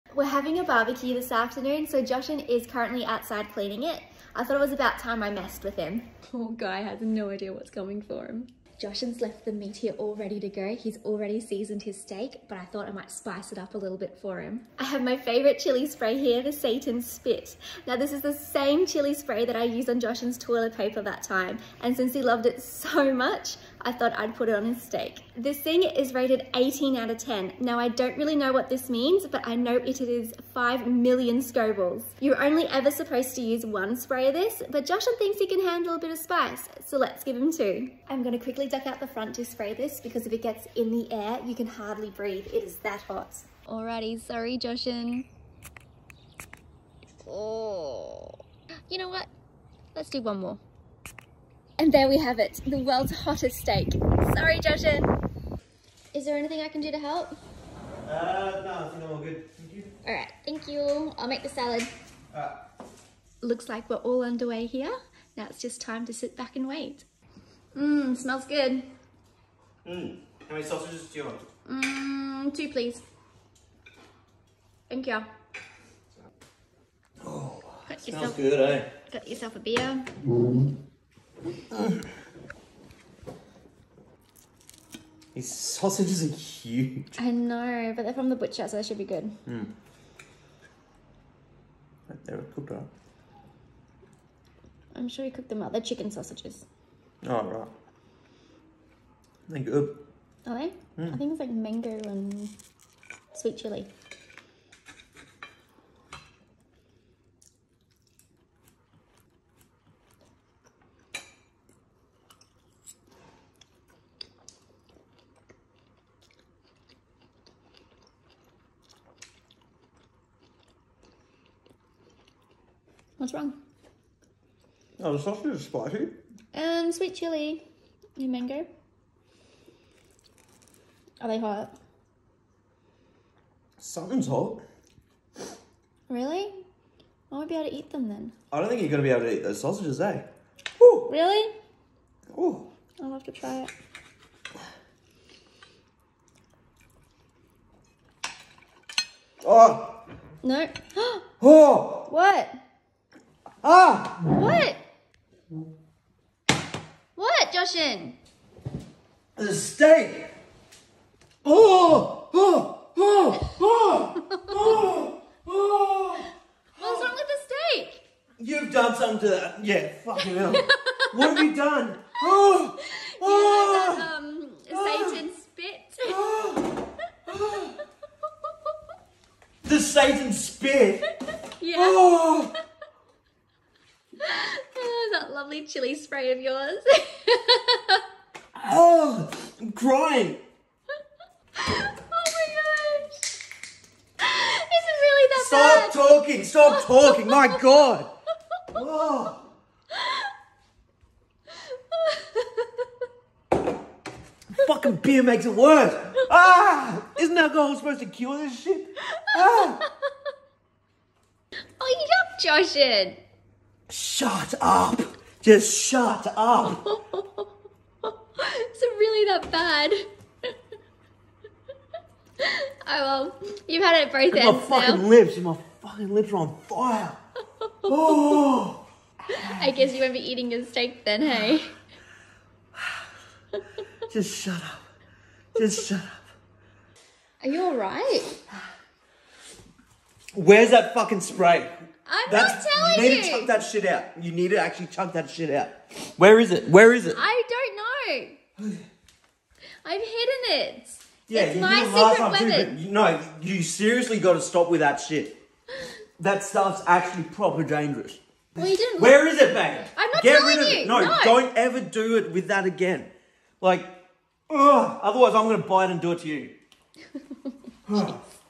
Spiciest Steak in the World Prank
Jashan and Cassandra Husband Vs Wife Couple Prank War:
Cassandra uses the chilli spray again, this time on one of a mans most prized posessions - his steak.